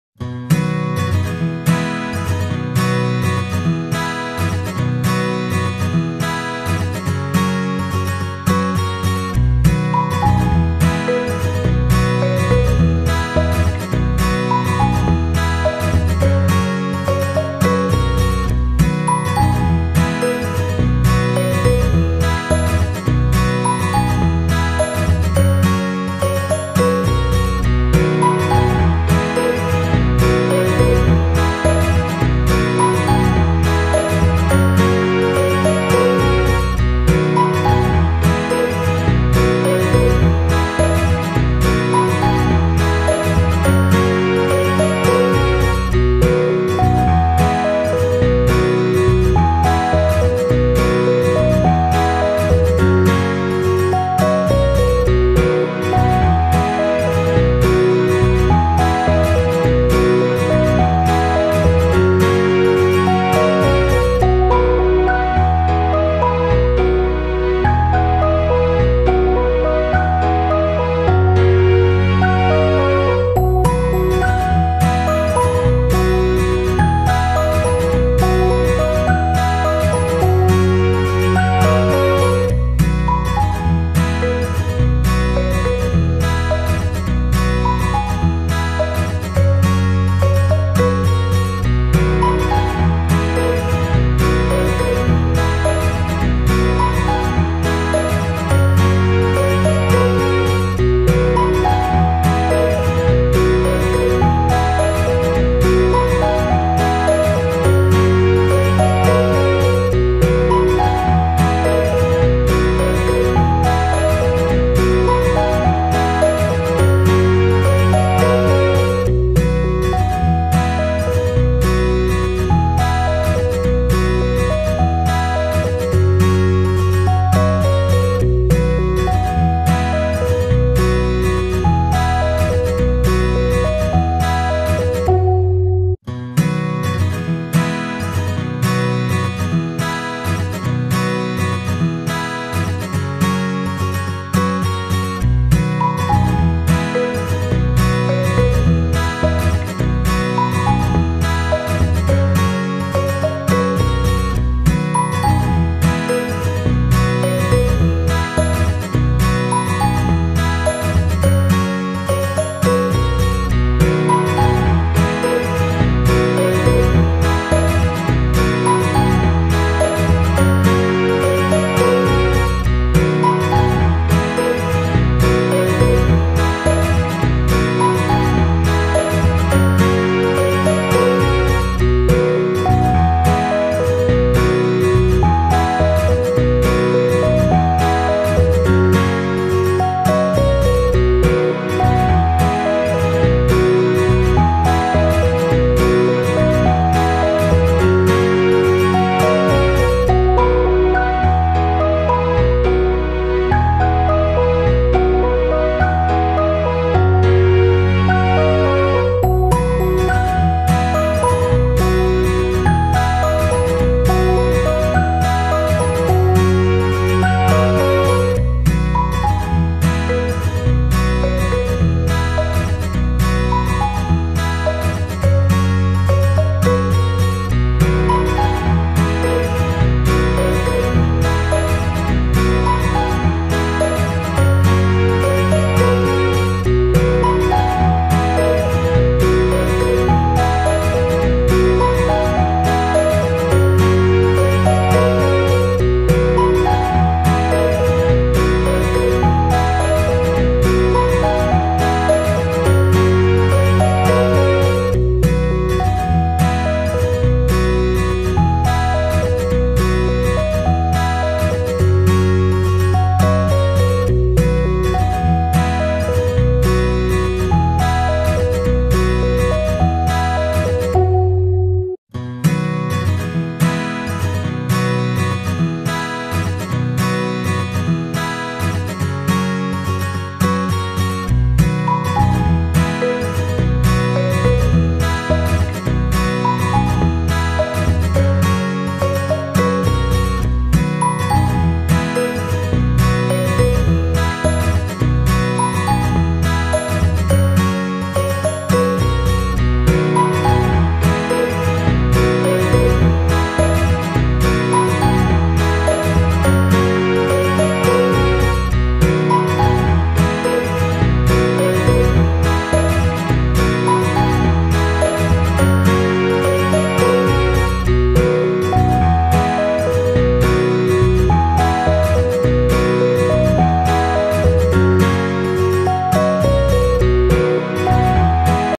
How to design your headbands at home/Home Made Vlog's
Great ideas to create your own cute headbands.